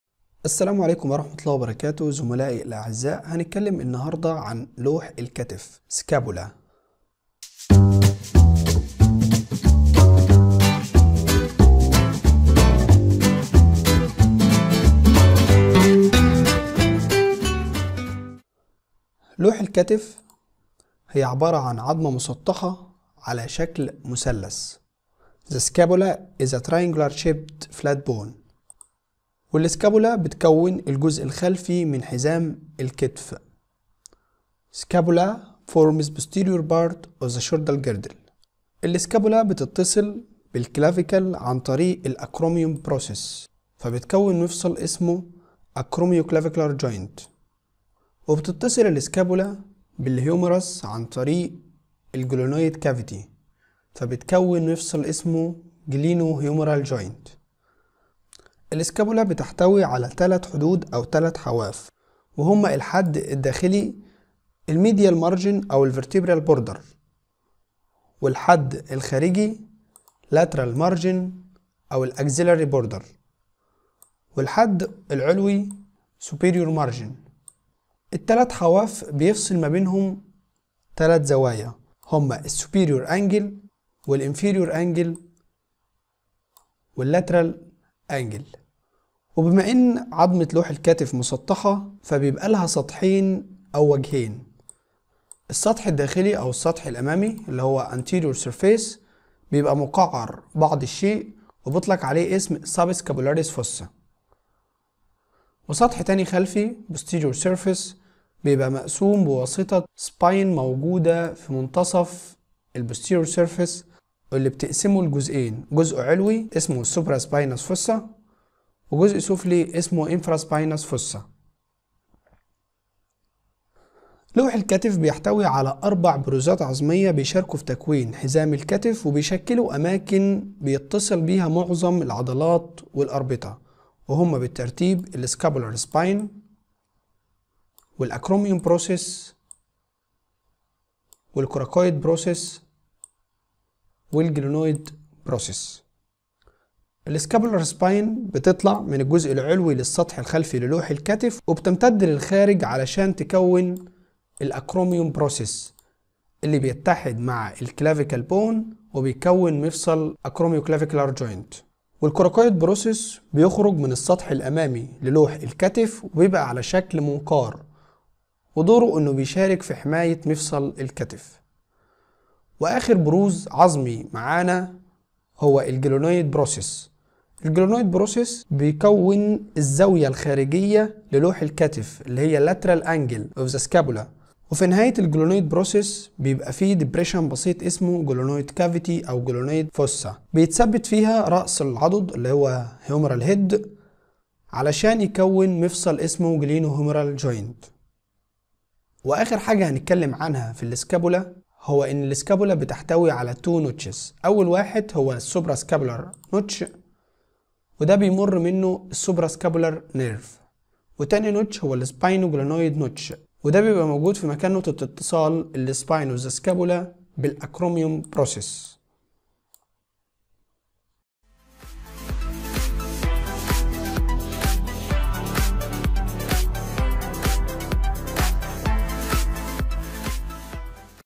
لوح الكتف || scapula 3d
وصف تشريحي ثلاثي الابعاد لعظمه لوح الكتف
Scapula
المختصر المفيد